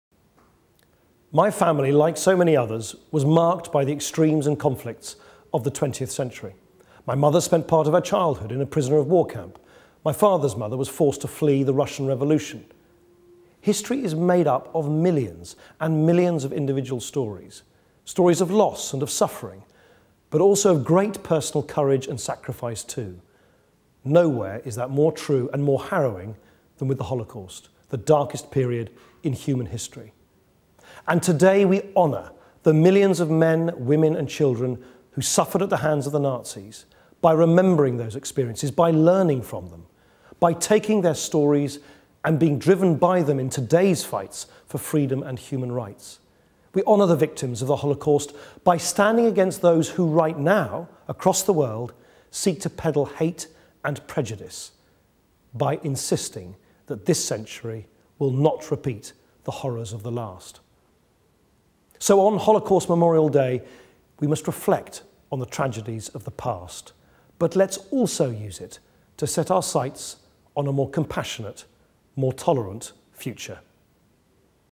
Holocaust Memorial Day
Message by Deputy Prime Minister Nick Clegg to mark Holocaust Memorial Day.